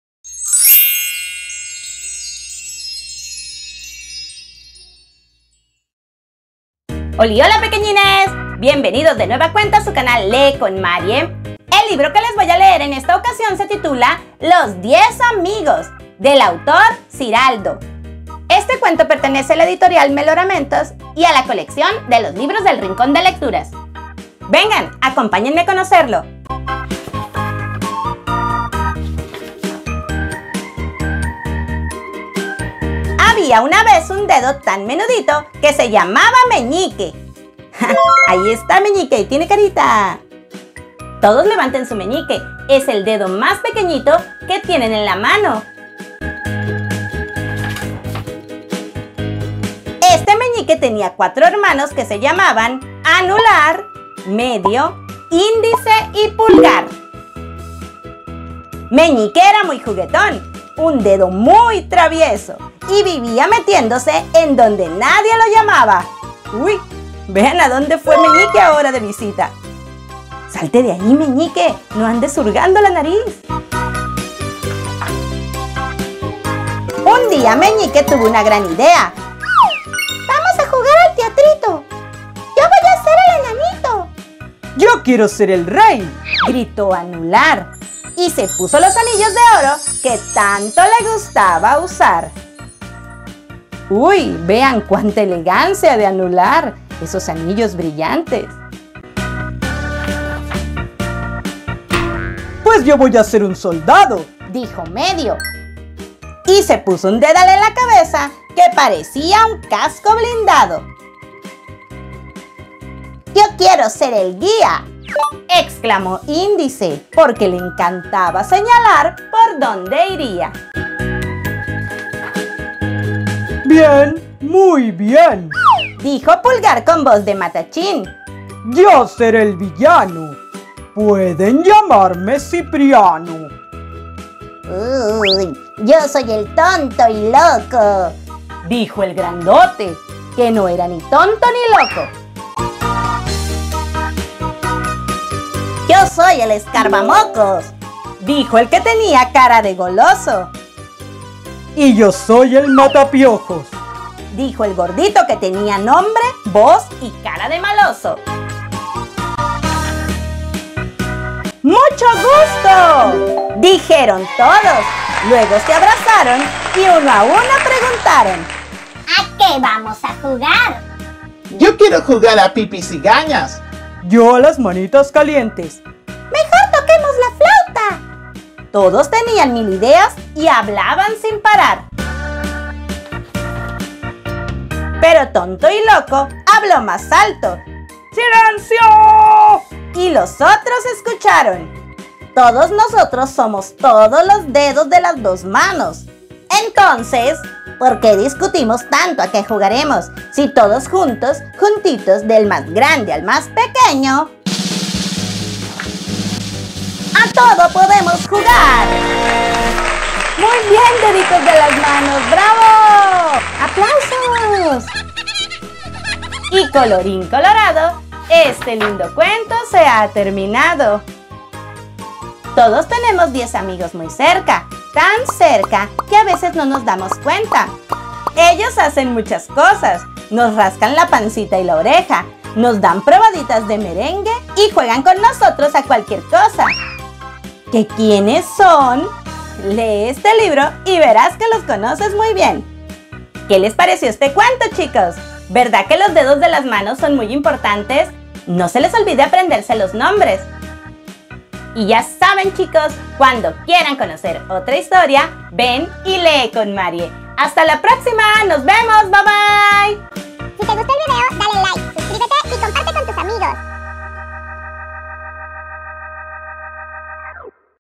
LOS DIEZ AMIGOS - Ziraldo - Lee con Marie
Todos tenemos diez amigos muy cerca; tan cerca, que a veces no nos damos cuenta.
Ellos hacen muchas cosas: nos rascan la pancita y la oreja, nos dan probaditas de merengue y juegan con nosotros a cualquier cosa.
¡¿Qué quiénes son?!
Lee este libro y verás que los conoces muy bien.